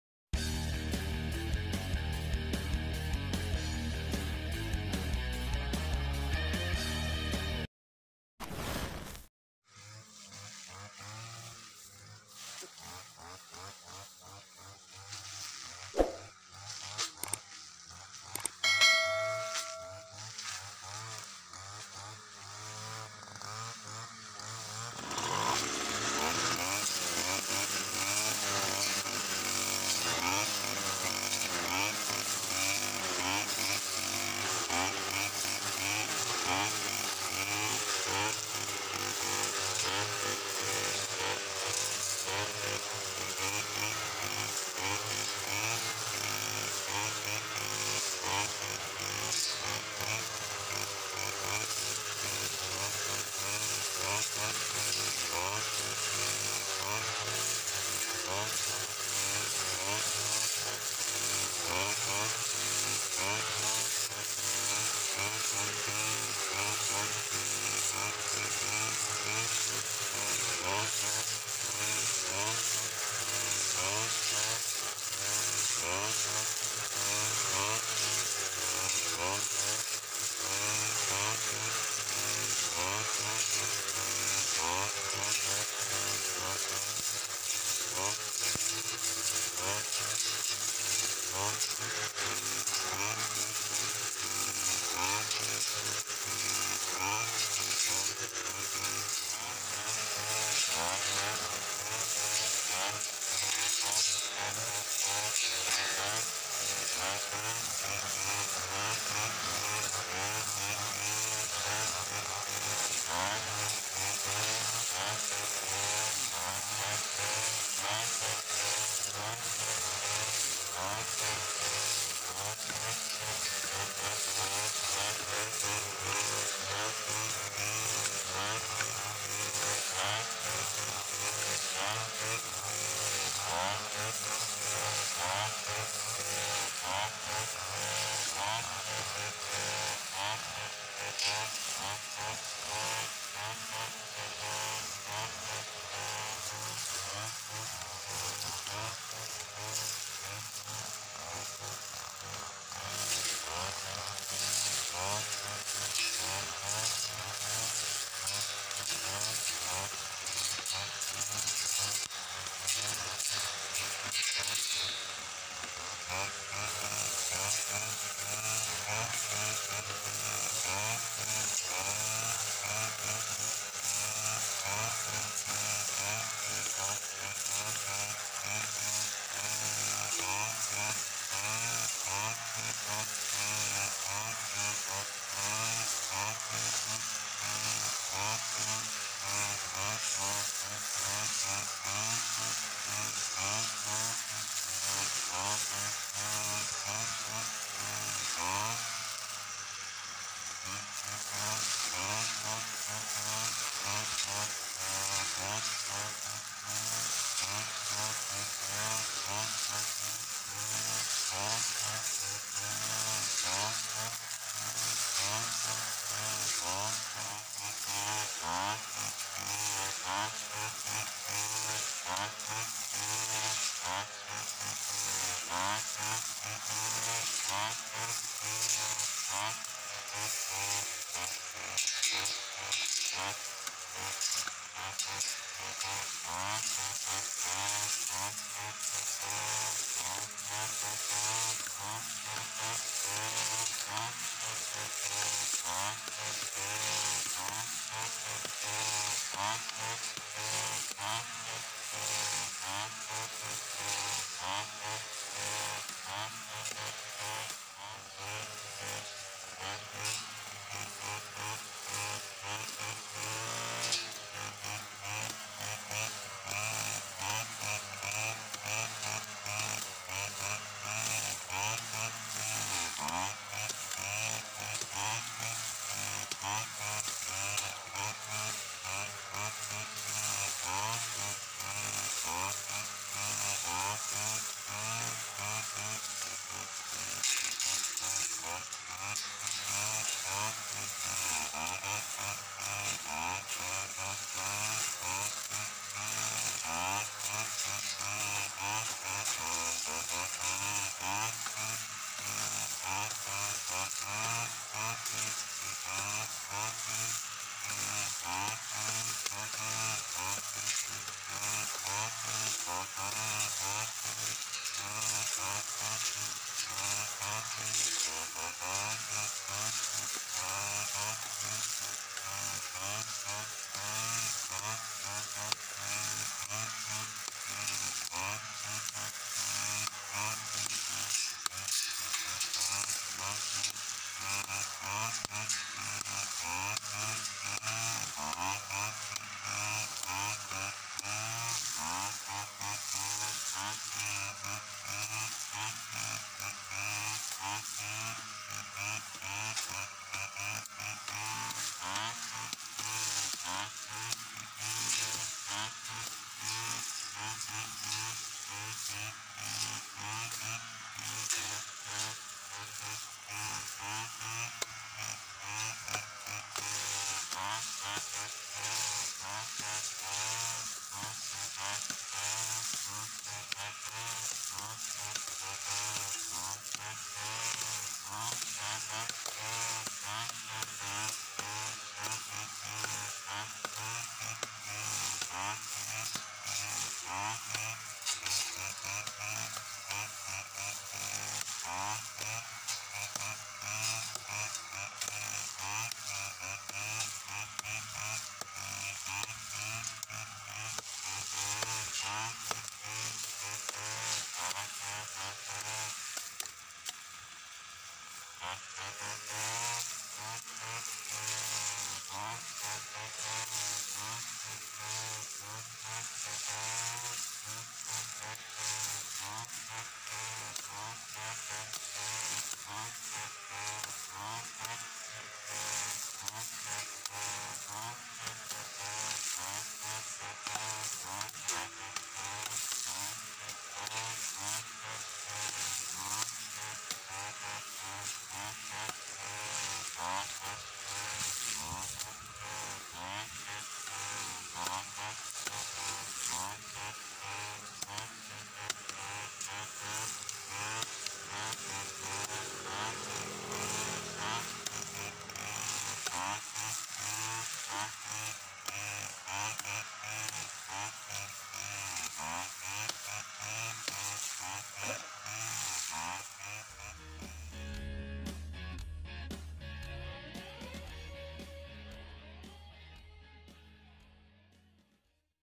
GRASS CUTTING 2 MONTHS OLD GRASS /Ground maintenance
About this video, I am cutting the 2 months old grass in the vacant property.